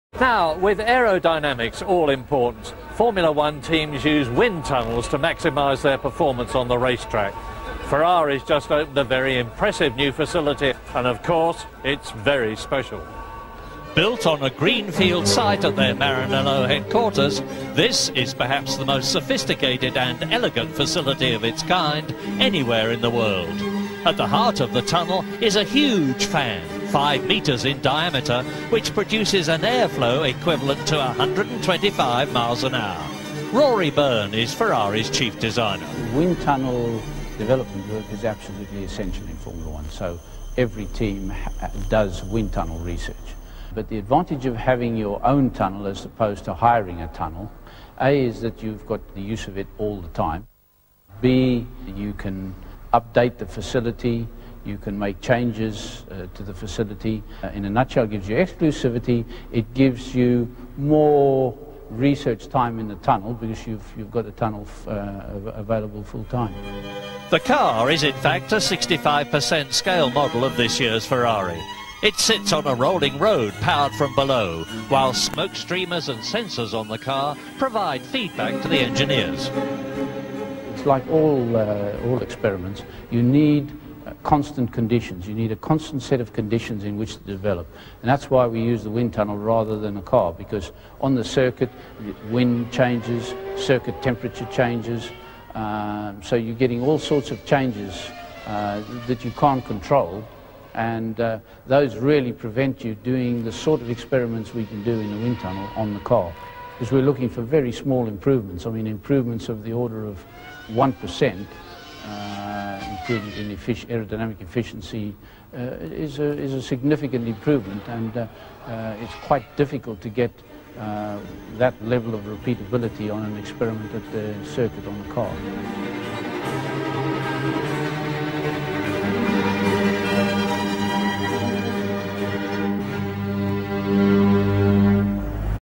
1998 F1 Hungarian GP - Ferrari wind tunnel visit
Following the crisis of the early nineties from 1996, Ferrari applied even more energy to the restructuring of the Formula 1 team. In July of that year, work began on the construction of the Scuderia's new wind tunnel. However, Ferrari wasn't going to cut corners and wanted a facility that could work with 1:2 scale models, 50% of actual size, but also 1:1, with full-size single-seaters and road cars. The work took a year and a half. The new wind tunnel can handle 1:2 models, simulating speeds approaching 250 km/h, or it can accommodate the real car itself (1:1 model) at speeds of up to 150 km/h. The electrical substation that feeds the system contains 6000 kW installations, enough power to light about 2000 apartments. The fan is about five metres in diameter and is equipped with a 2200 kW turbine, the equivalent of five Formula 1 engines. The instrumentation has a maximum margin of error of 0.04%, and it cost more than 16 billion lire, about 8 million euros. The facility opened in 1998 and is mainly used for the development of F1 cars. The F300 of that year only partially took advantage of that system, while the first car to make full use of the new wind tunnel was the F399 that, in 1999, won the Constructors' title.